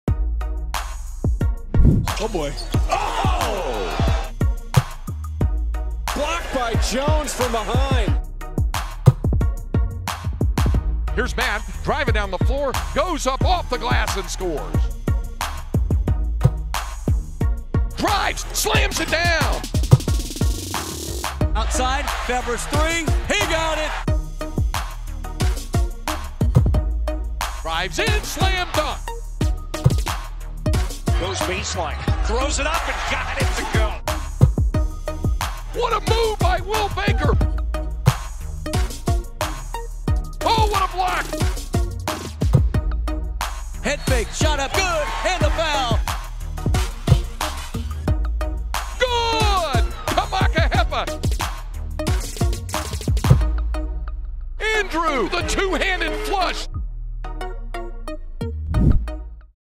Texas Men's Basketball 2020-21 Non-Conference Schedule [July 2, 2020]
Home game against Gonzaga and Maui Jim Maui Invitational appearance highlight UT’s non-conference slate.